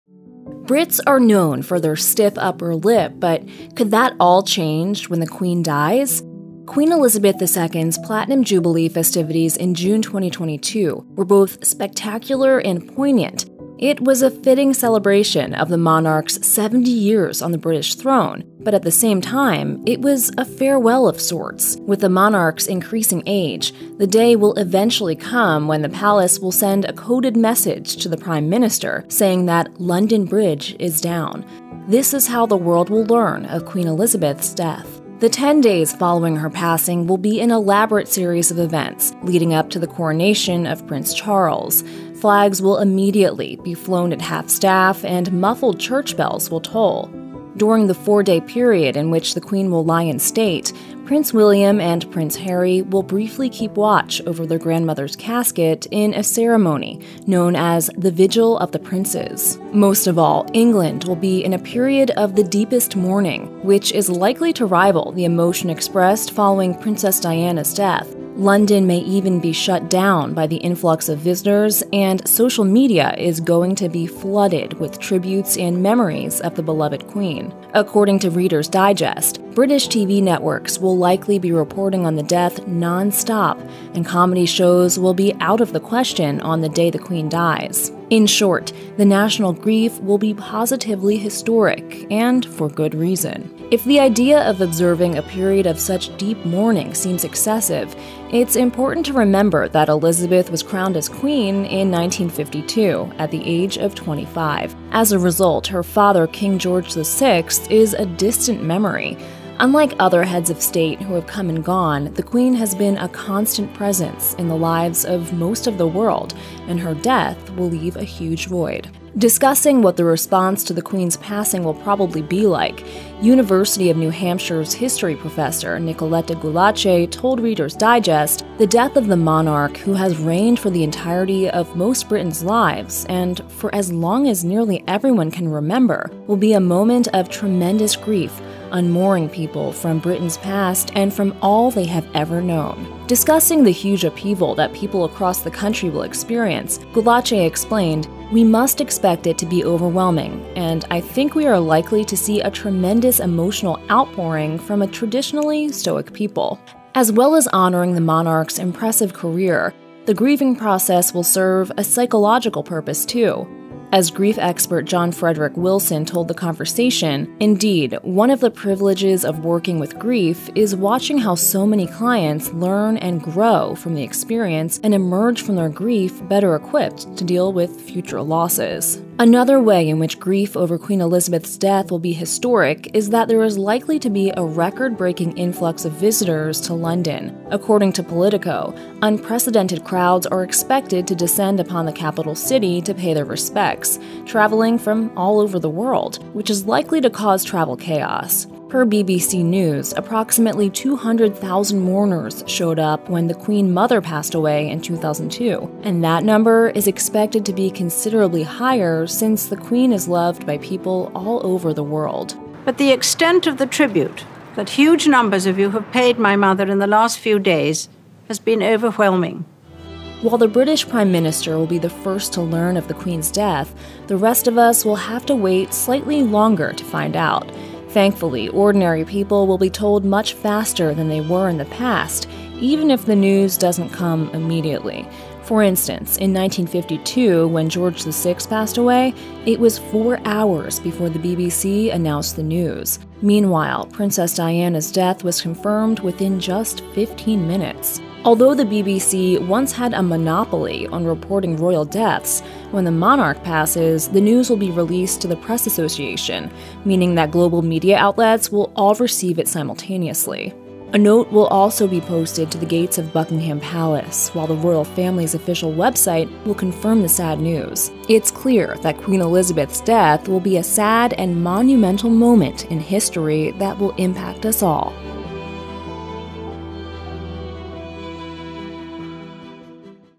Why British Grief Over The Queen's Death Will Be Historic
Brits are known for their stiff upper lip, but could that all change when Queen Elizabeth II dies? After her long reign, people all over the world are set to mourn the beloved monarch, but here's why British grief will be historic.